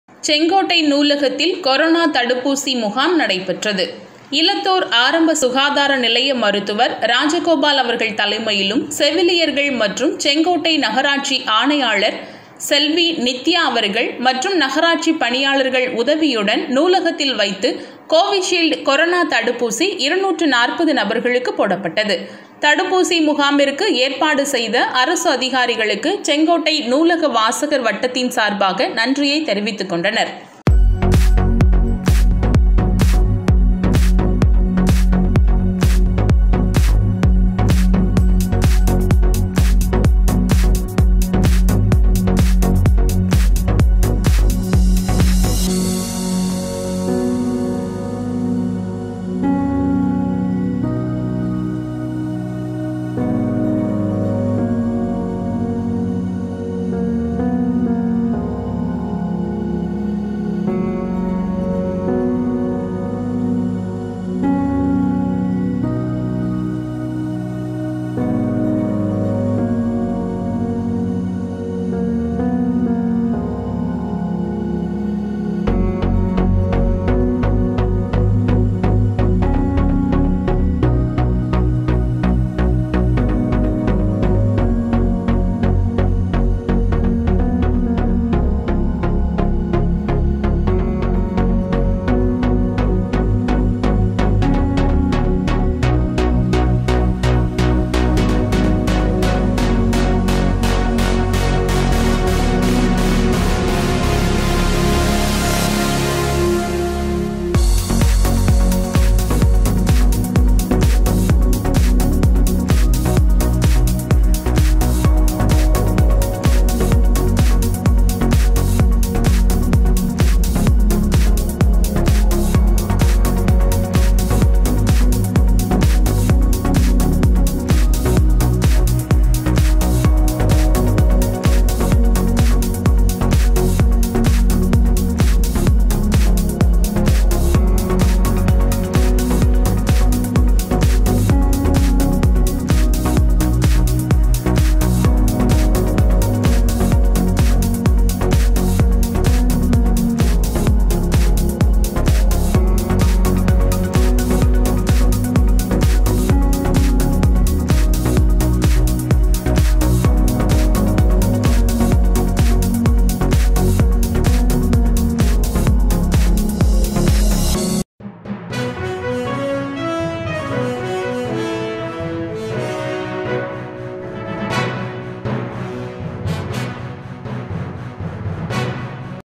செங்கோட்டை நூலகத்தில் கொரானா தடுப்பூசி முகாம் | MAYURI-TV | MAYURI NEWS | TAMIL
செங்கோட்டை நூலகத்தில் கொரானா தடுப்பூசி முகாம்
இலத்தூர் ஆரம்ப சுகாதார நிலைய மருத்துவர். ராஜகோபால் அவர்கள் தலைமையிலும் செவிலியர்கள்  மற்றும் செங்கோட்டை நகராட்சி ஆணையாளர்  செல்வி. நித்யா அவர்கள் மற்றும்  நகராட்சி பணியாளர்கள் உதவியுடன்  நூலகத்தில் வைத்து  கோவிஷீல்டு
 கொரானா தடுப்பூசி 240 நபர்களுக்கு
போடப்பட்டது.
      தடுப்பூசி முகாமிற்கு ஏற்பாடு செய்த அரசு அதிகாரிகளுக்கு செங்கோட்டை நூலக வாசகர் வட்டத்தின் சார்பாக நன்றியை தெரிவித்துக் கொள்கிறோம்.